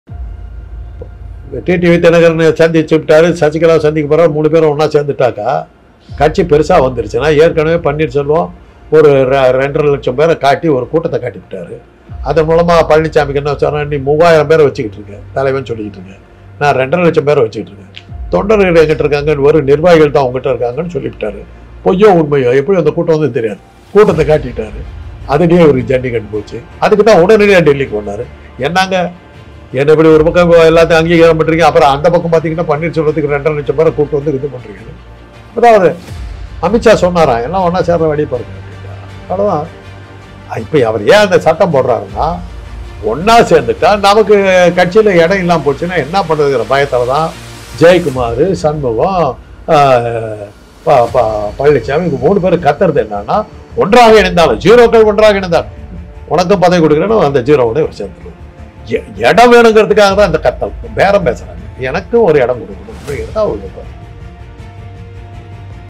அதிமுக ஒன்று படவிடக்கூடாது என்று நினைப்பவர்கள் எடப்பாடி பழனிசாமி, ஜெயக்குமார், etc
அதிமுக ஒன்று படவிடக்கூடாது என்று நினைப்பவர்கள் எடப்பாடி பழனிசாமி, ஜெயக்குமார், சி. வி. சண்முகம்...